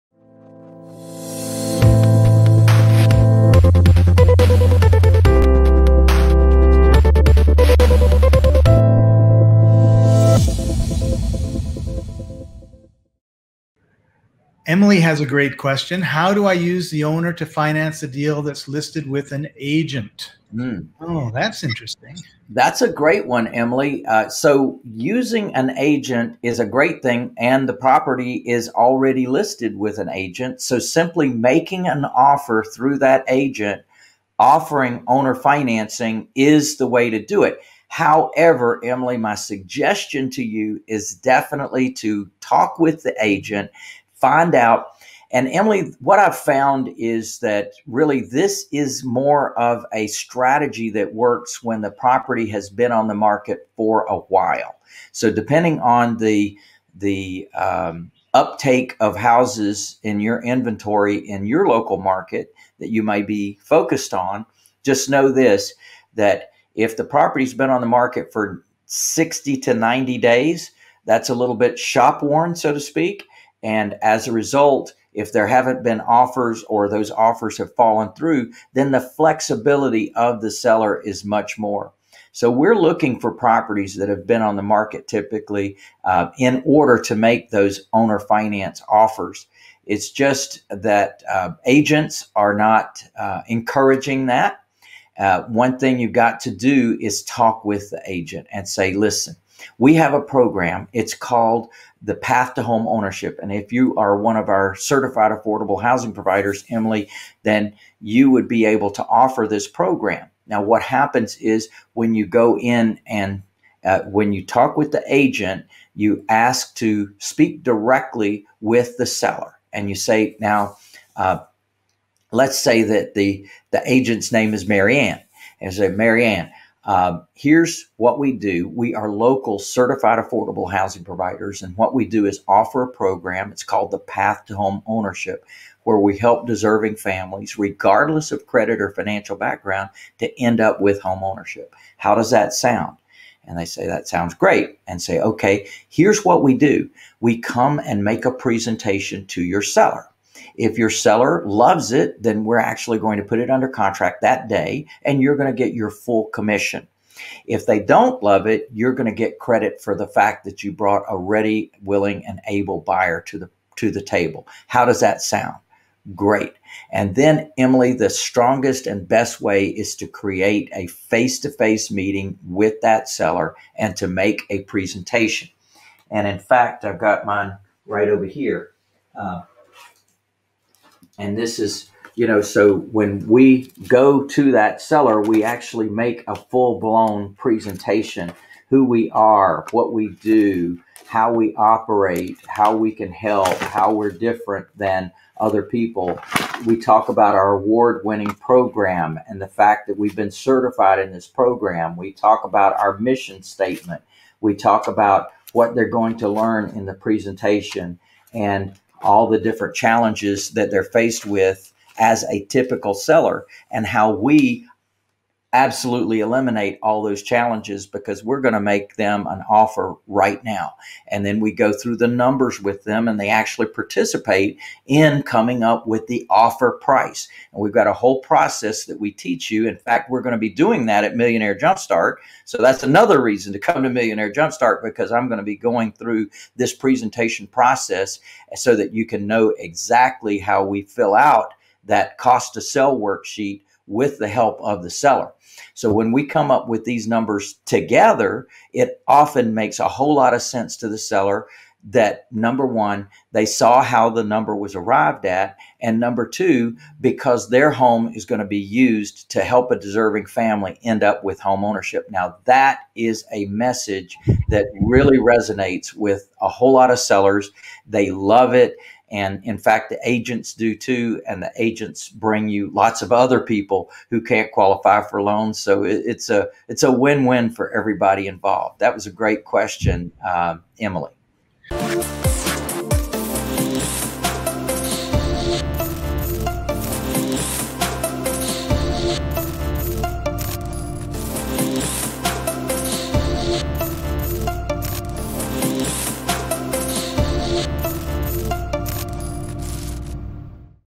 How to Owner-Financed a Deal with Real Estate Agent
How will you use an owner-financed strategy with a real estate agent?

Watch this short video now and find out Lou’s answer.

For more valuable information click on this link and watch the complete episode - “Chaos In The Real Estate Market or Hype?”